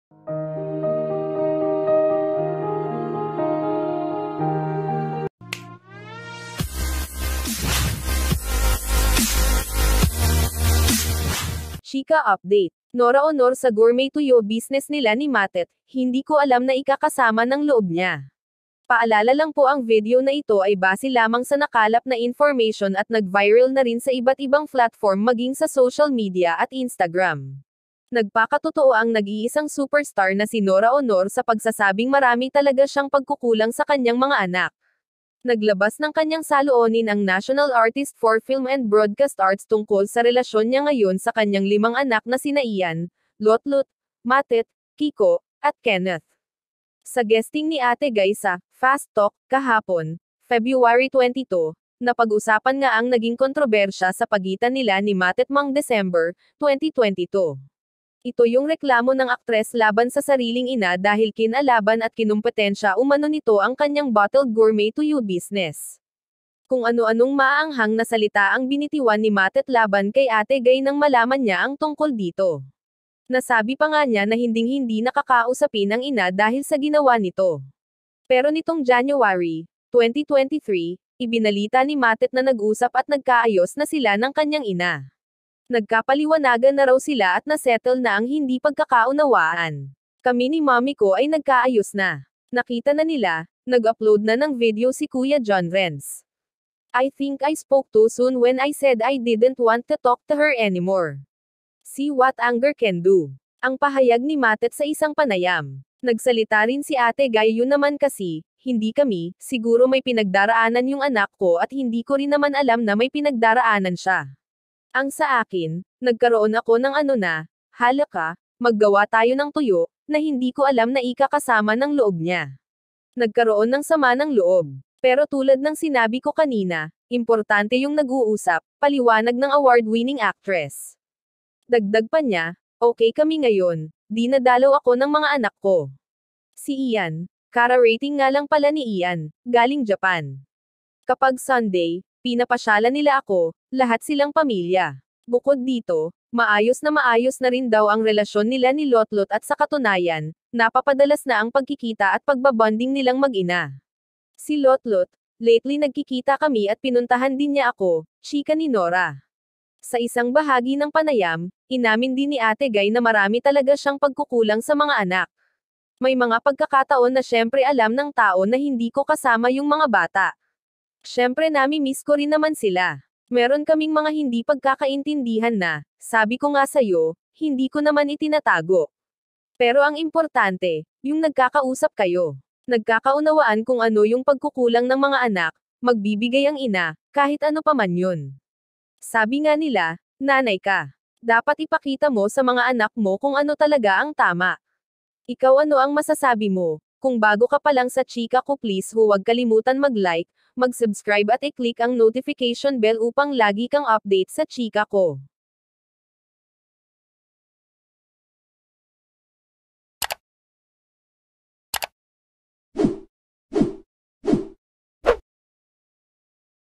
NAKAKAIYAK:Reaksyon ni Matet De Leon sa paghingi ng tawad ni Nora Aunor!
Nakakadurog Ng Puso Matet Nangiyak-ngiyak!

In this Video want to share the latest update IN SHOWBIZZ WORLD

Viral Today's!